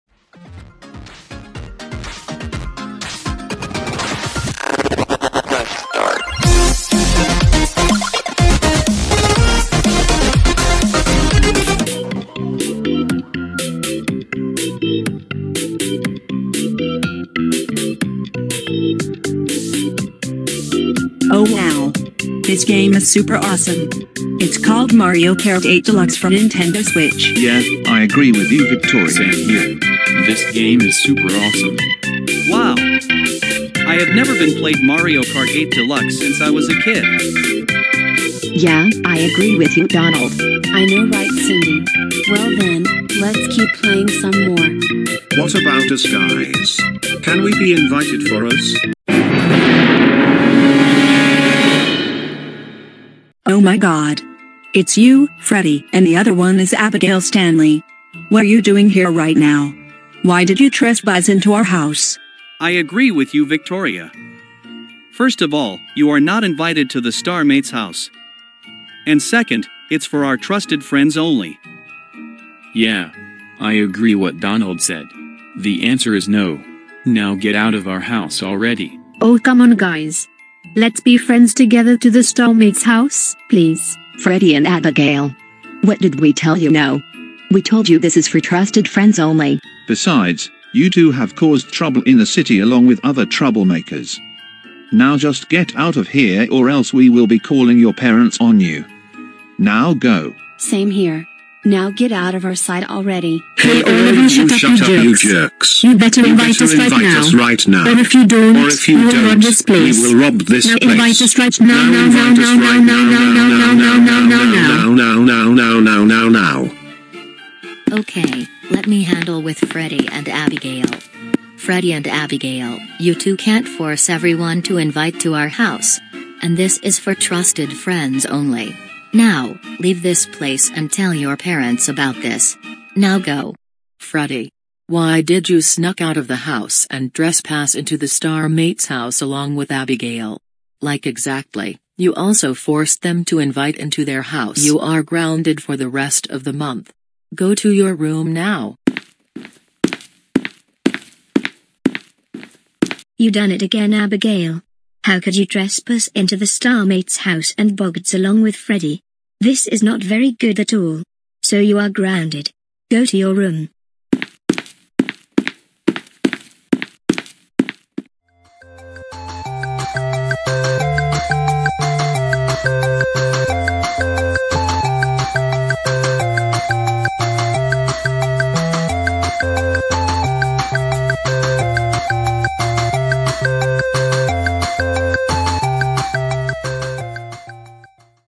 Freddy And Abigail Barges To The Star Mates House/Grounded
In The Star Mates House, they play the games at the TV, but then Freddy And Abigail forcing them to be trusted friends and barges them.

Program used:
Windows 11
OBS Studio
Wrapper Offline 2.2.5
CapCut